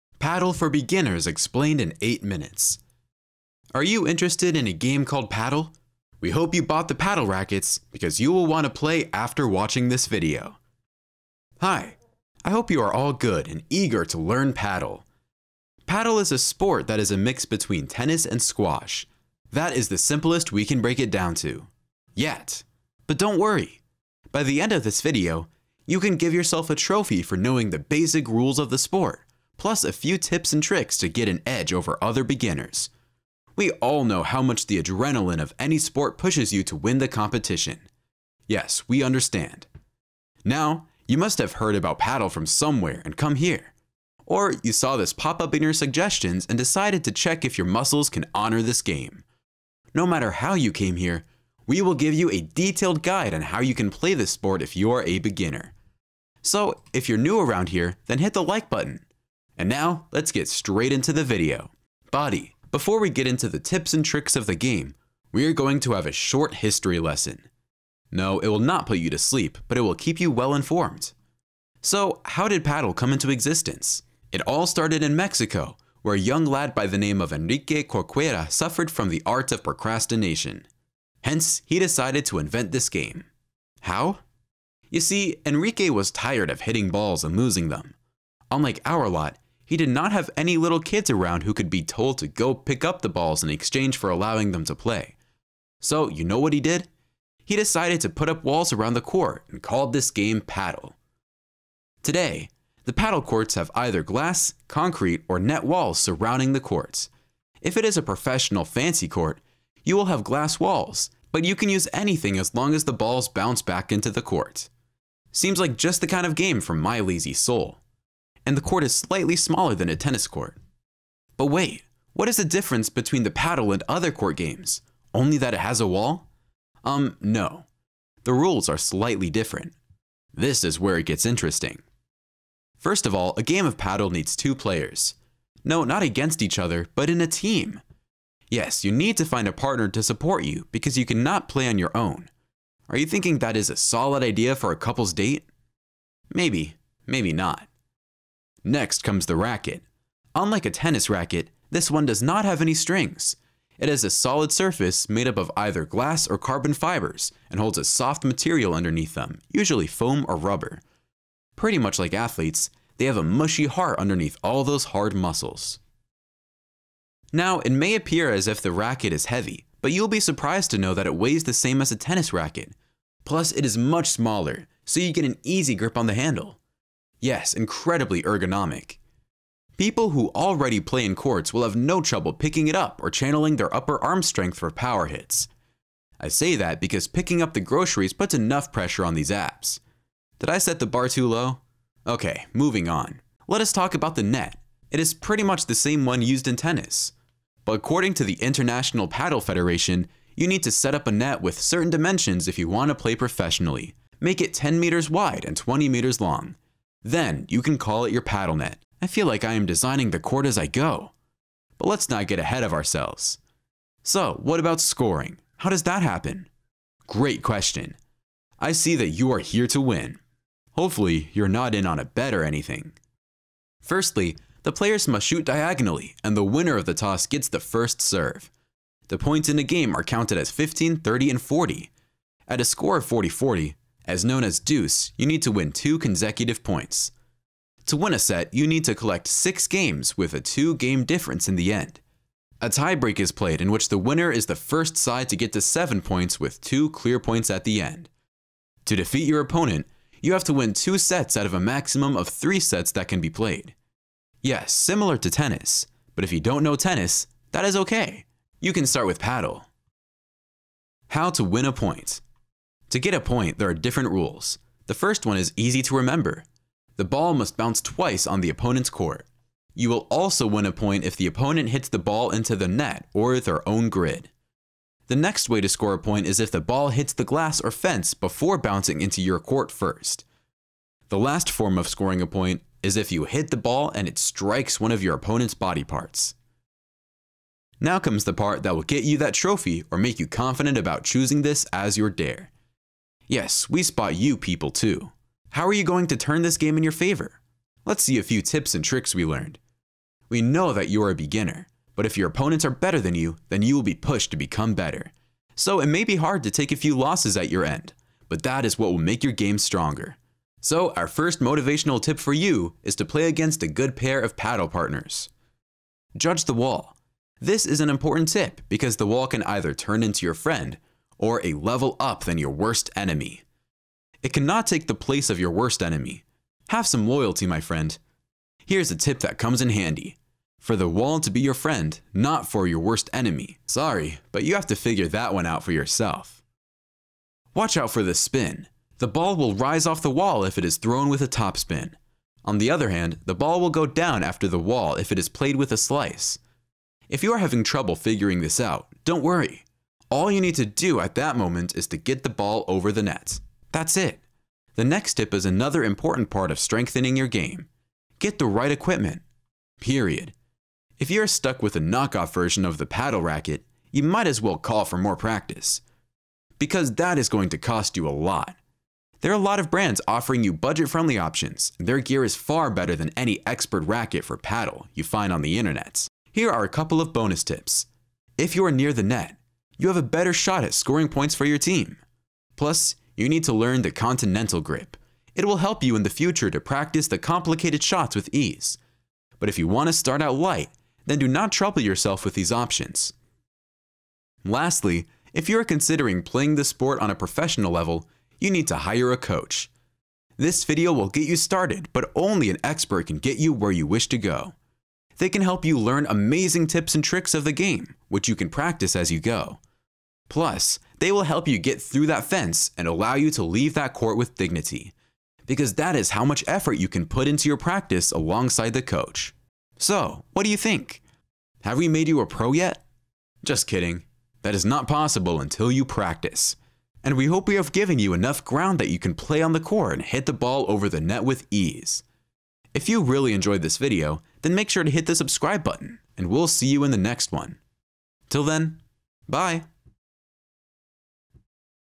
Padel EXPLAINED for beginners in 7 minutes | All Rules
Hi, I hope you are all good and eager to learn padel!
Padel is a sport that is a mix between tennis and squash. That is the simplest we can break it down to! Yet. But don’t worry! By the end of this video, you can give yourself a trophy for knowing the basic rules of the sport plus a few tips and tricks to get an edge over other beginners! We all know how much the adrenaline of any sport pushes you to win the competition! Yes, we understand.

Now, you must have heard about padel from somewhere and come here. Or you saw this pop up in your suggestions and decided to check if your muscles can honor this game! No matter how you came here, we will give you a detailed guide on how you can play this sport if you are a beginner.